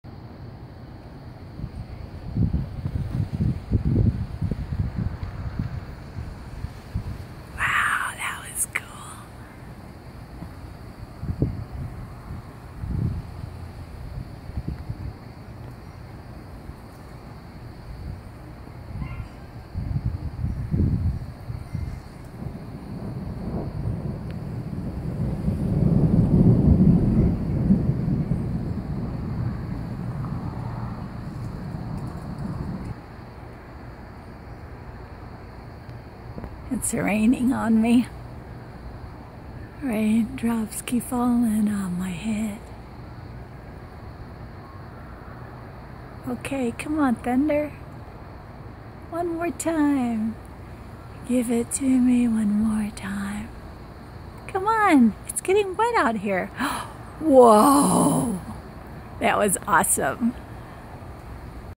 Produce rain and thunder 6 10 2015 in bakersfield
A rare moment in California and totally unexpected. It rained for about an hour in Bakersfield, CA with thunder and lightning. Perhaps the drought will be over this year. One can only hope... In the meantime, if it's yellow, let it mellow. LOL ...